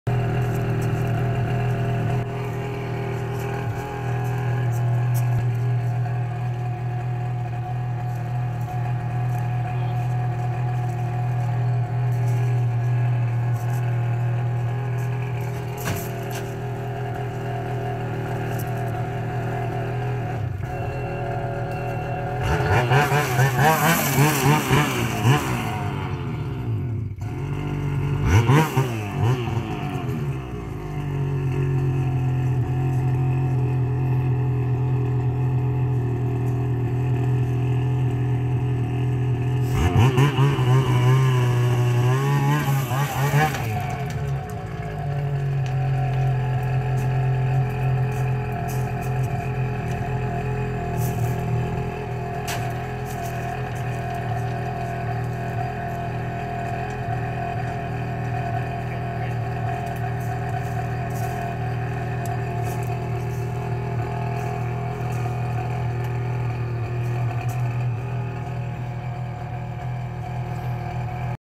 Tacoma Sled Loader
Pretty simple design using the reciever to prevent any weight on the tailgate. 2018 summit 850, 2019 Toyota Tacoma